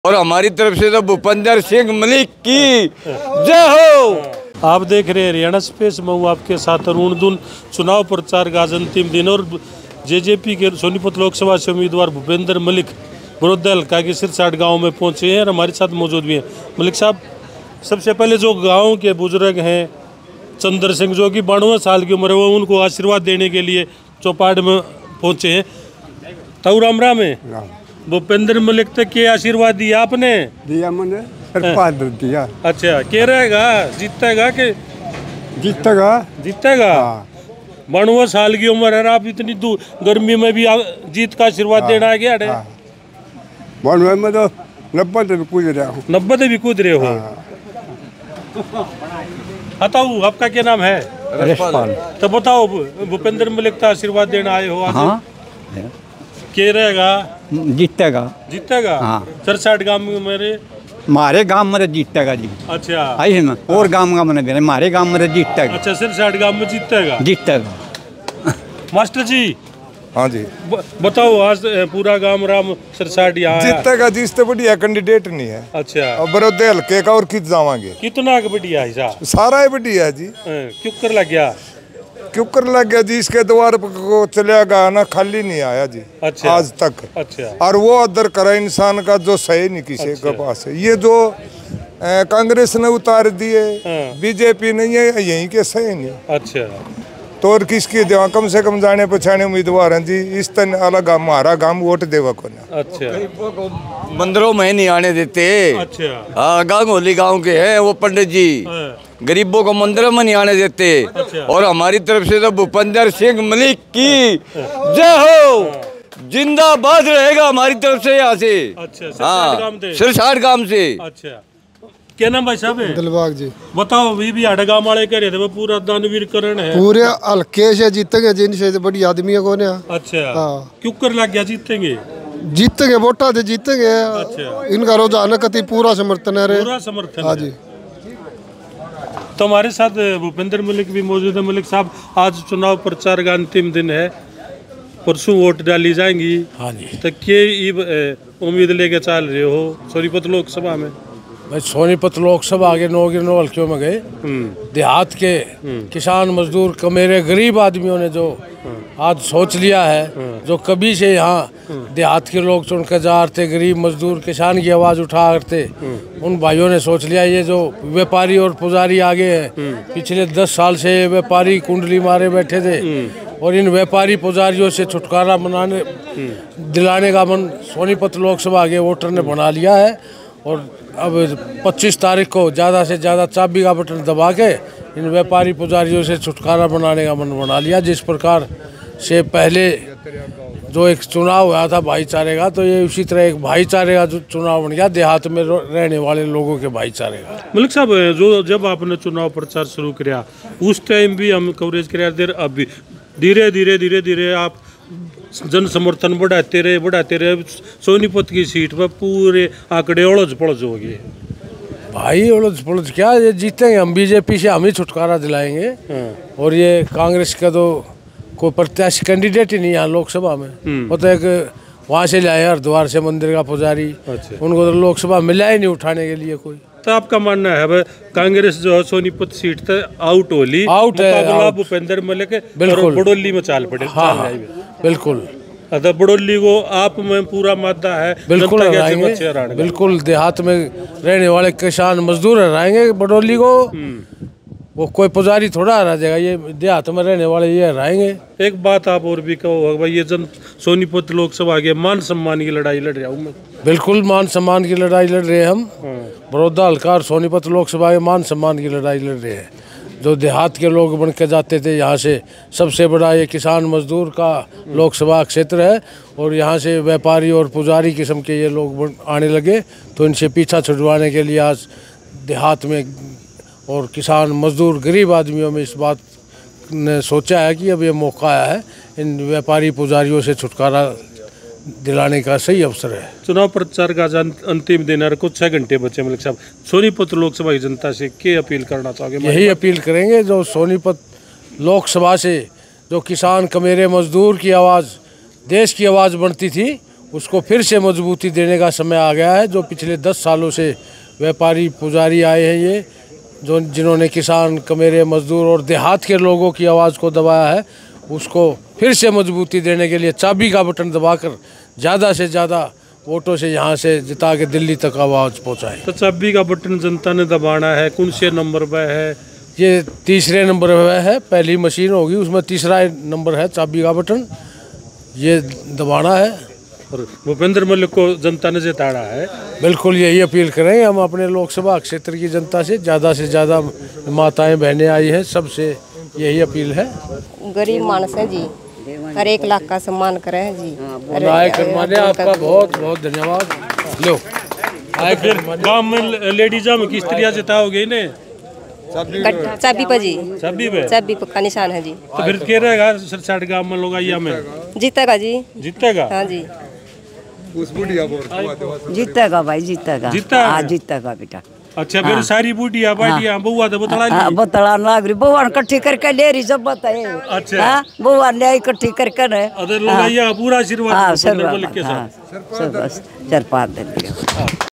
92 साल के ताऊ ने दिया भूपेंद्र मलिक को जीत का आशीर्वाद।
92 साल के ताऊ ने दिया भूपेंद्र मलिक को जीत का आशीर्वाद।